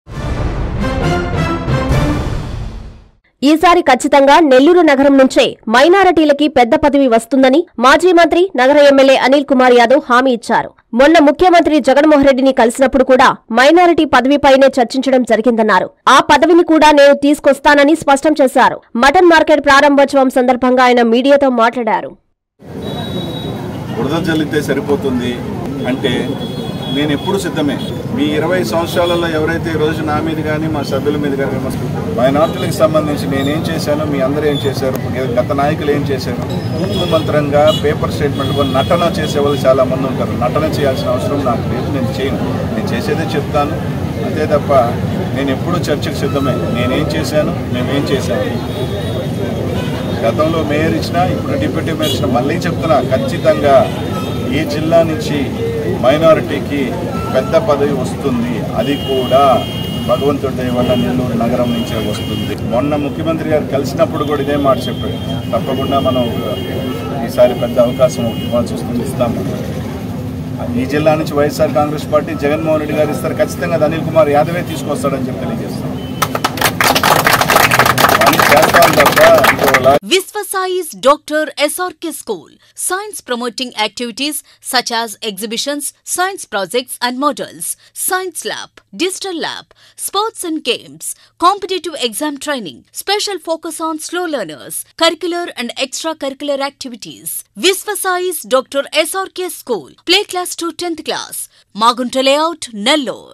ఈ సారి మైనార్టీకి పెద్ద ప‌ద‌వి...నెల్లూరు న‌గ‌రానికే
- మాజీ మంత్రి అనిల్ కుమార్ యాద‌వ్‌